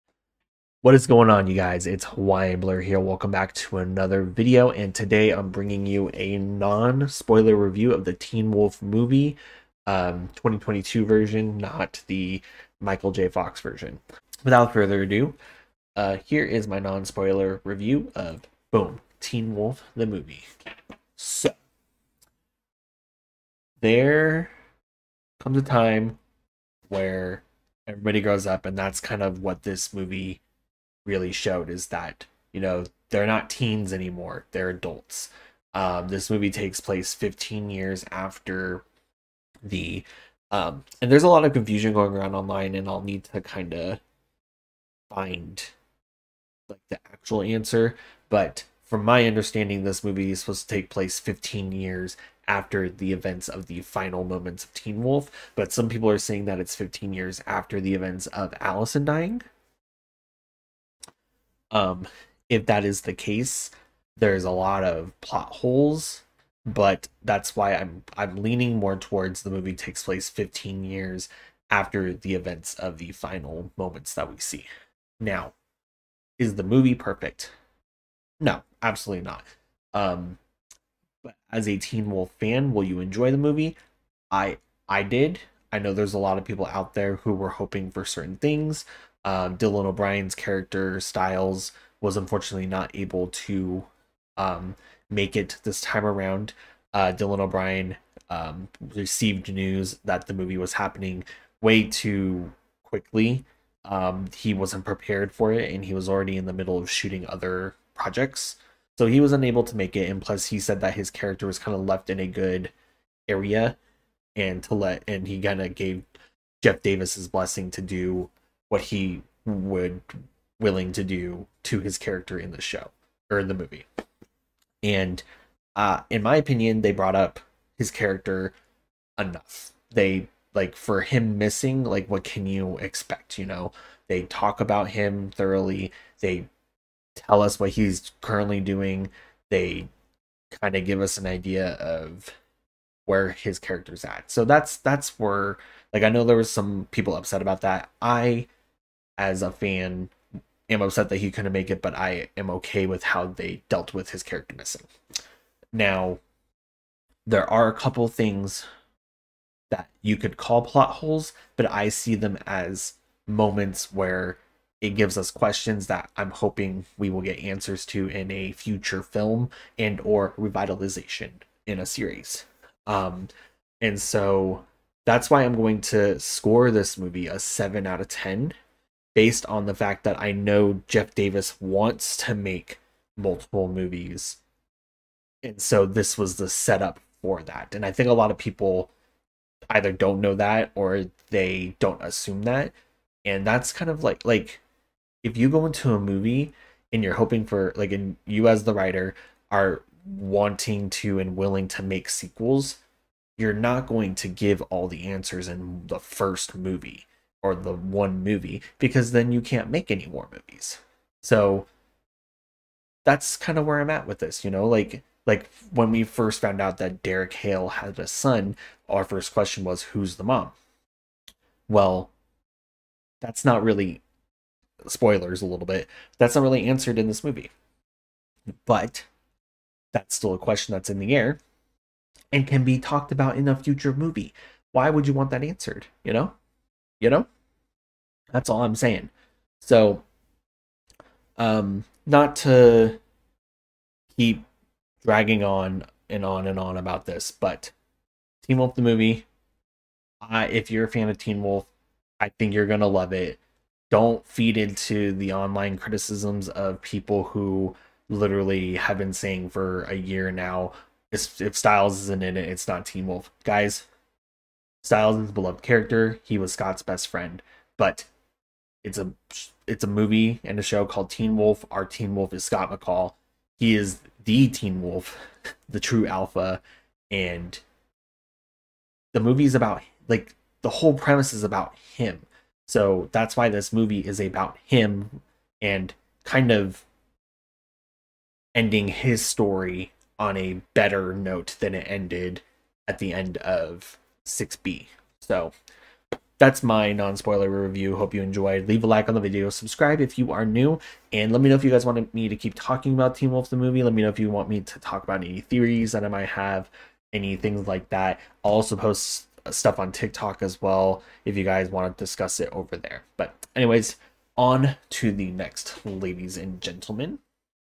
TEEN WOLF: THE MOVIE | Non-Spoiler Review!
The Teen Wolf Movie is finally here! And I’m here to give you my non-spoiler review!

Spoiler discussion during Tuesdays upcoming stream!

Teen Wolf the movie follows Scott McCall 15 years after the final events of Teen Wolf Season 6B. An old threat emerges, causing Scott to get the pack back together and return to Beacon Hills once more. With twists and turns throughout, will Scott continue to lead his pack as the True Alpha?